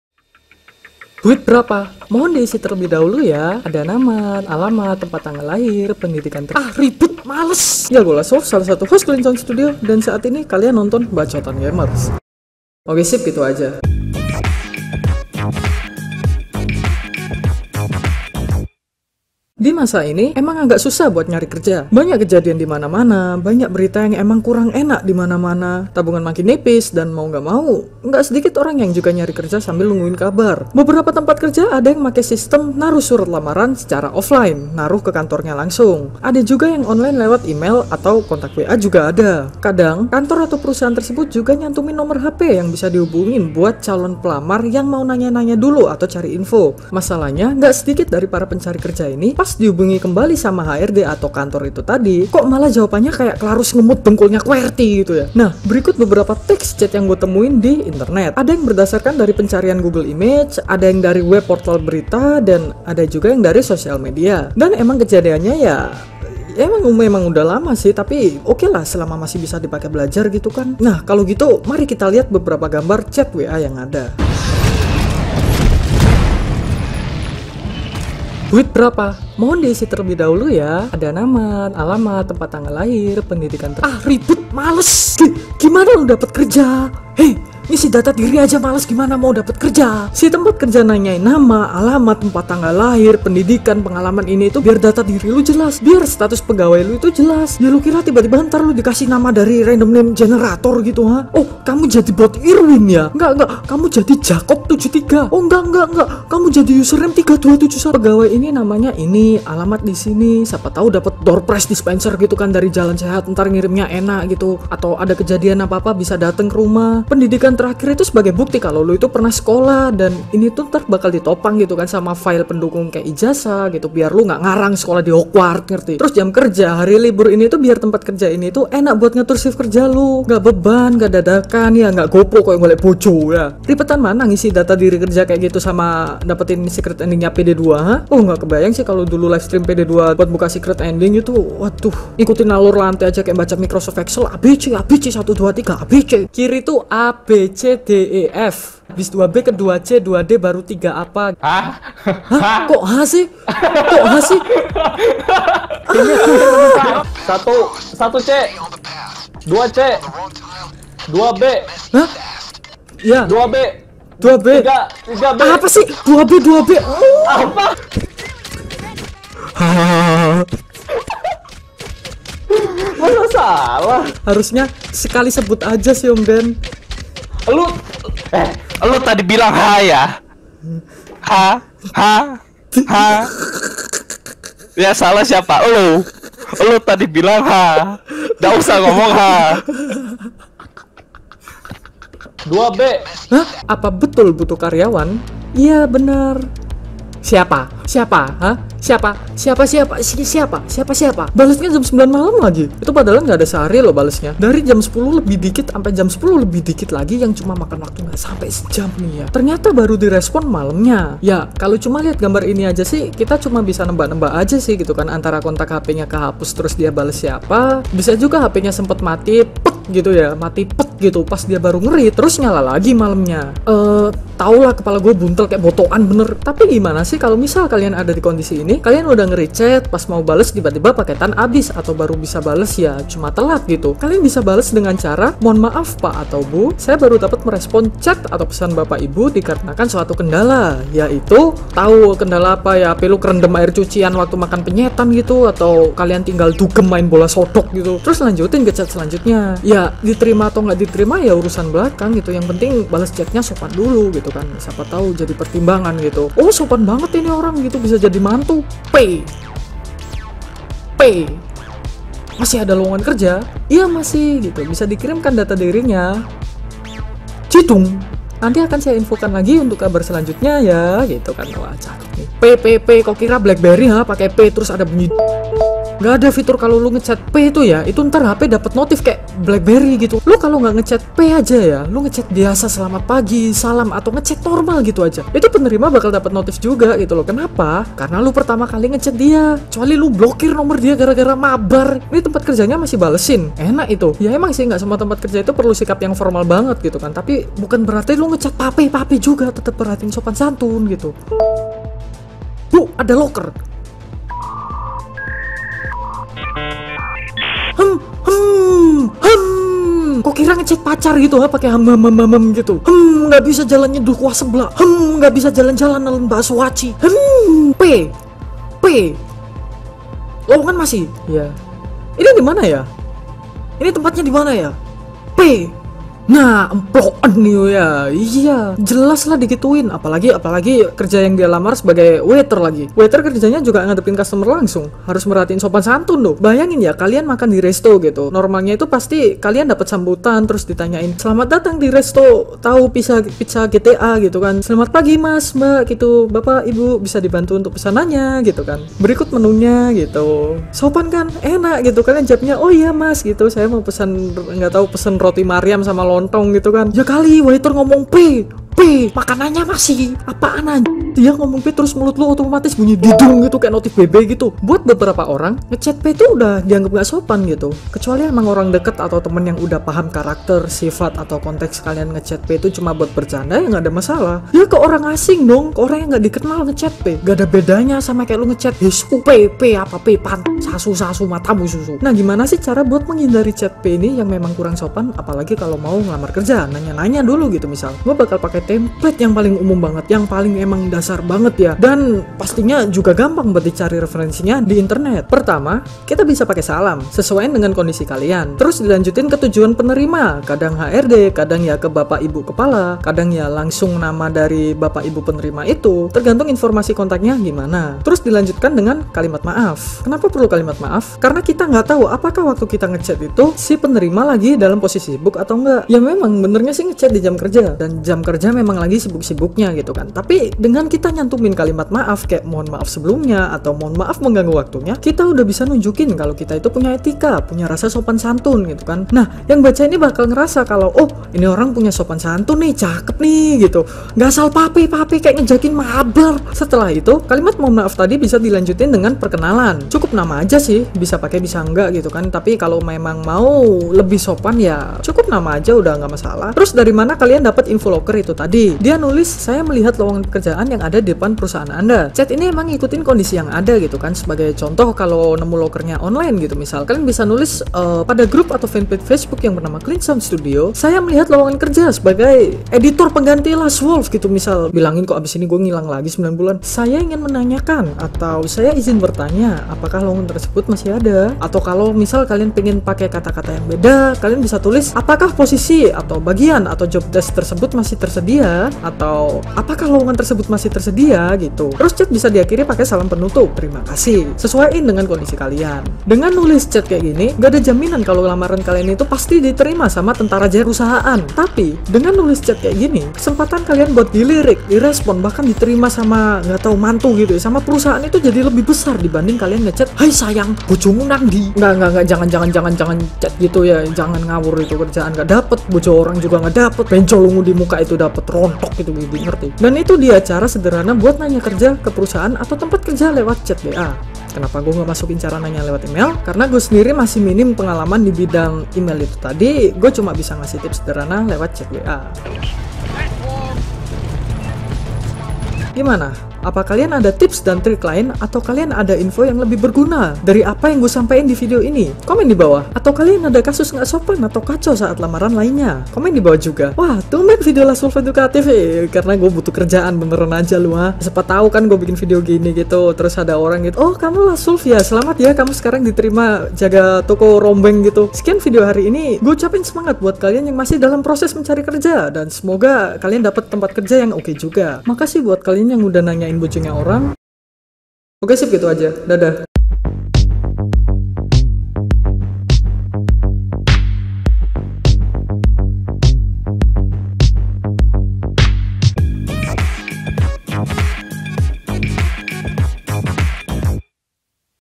Kumpulan Chat Lamaran Kerja Yang Bikin Malu
Mau lamar kerja tapi kok gini amat ya isi chatnya. Tidak ada sopan santun.

Untuk info mengenai endorsment, sponsorship, dan informasi lainya. Harap hubungi kami di:
Cindy Manager Cleansound Studio: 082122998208